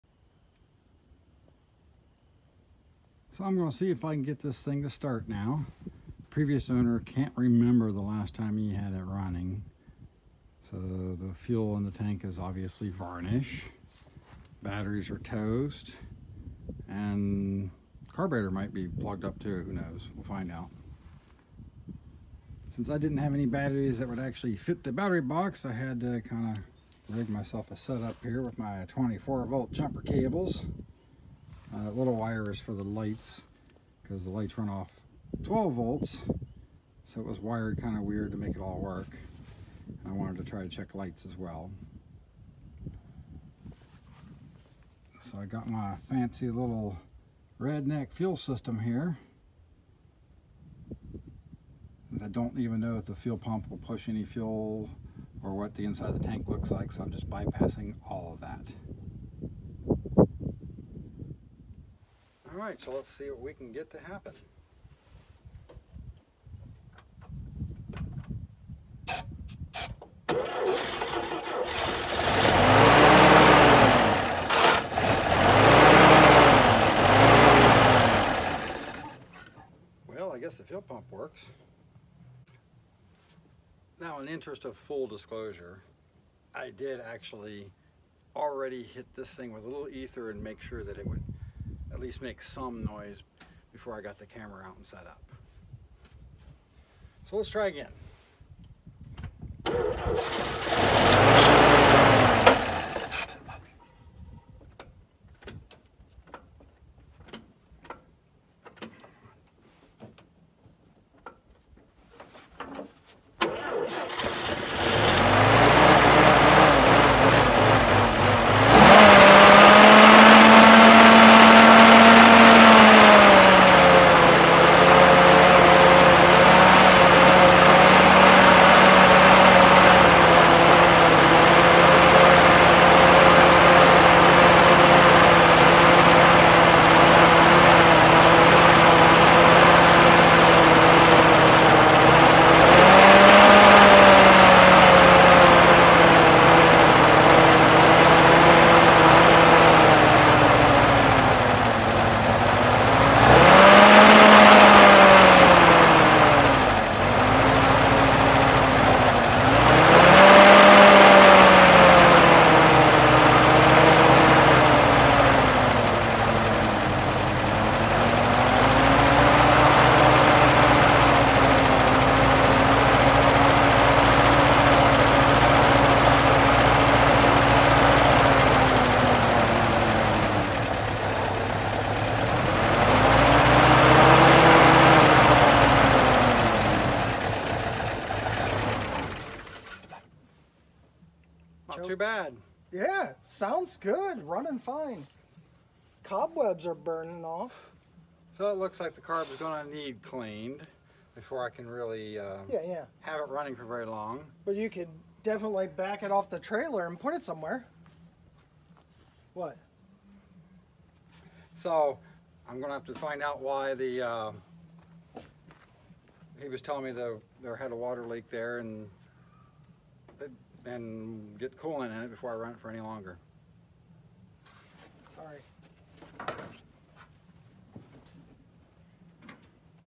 Getting the M38A1 running
Initially getting the Jeep running to assess it